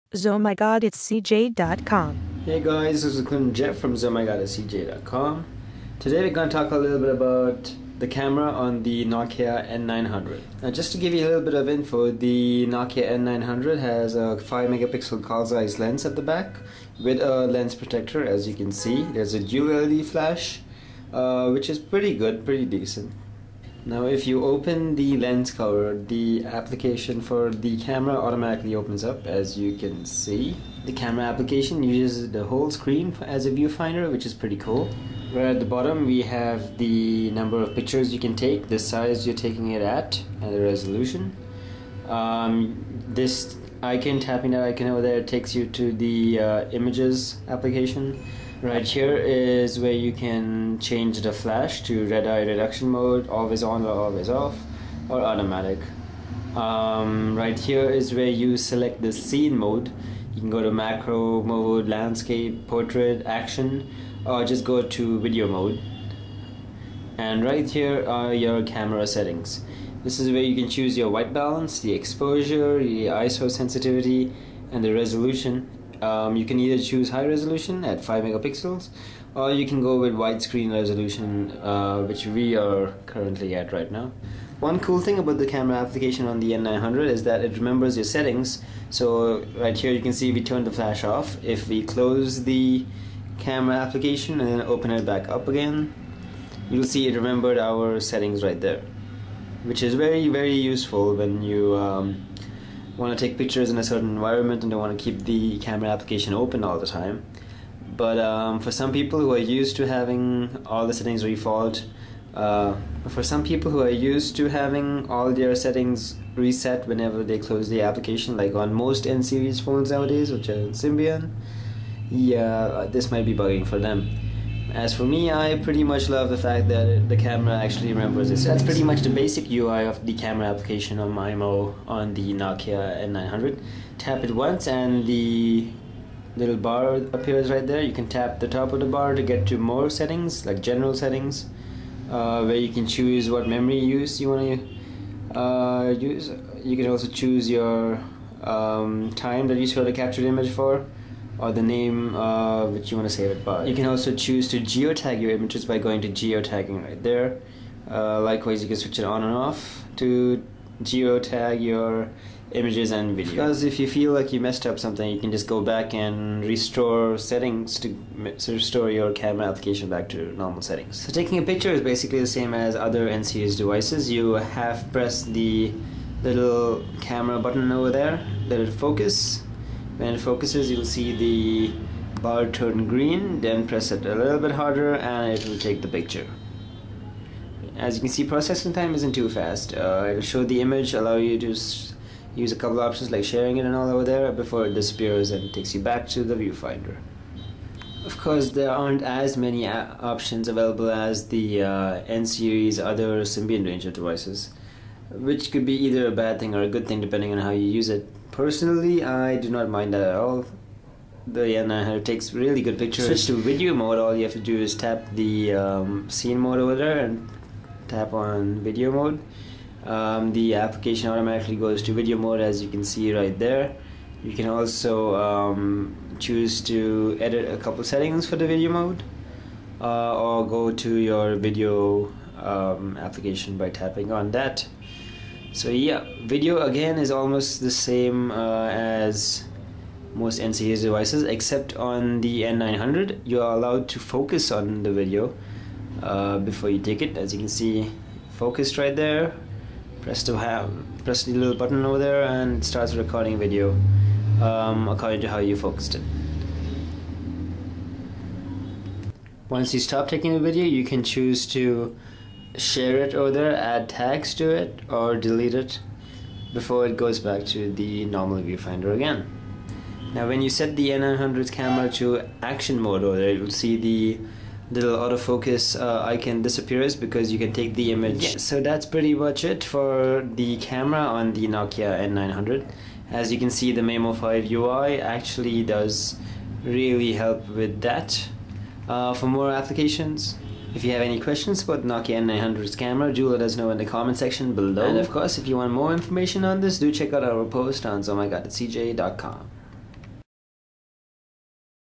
Nokia N900 Review : Camera on Maemo 5
Here's a short overview of the Camera application on the Maemo 5 OS, on a Nokia N900.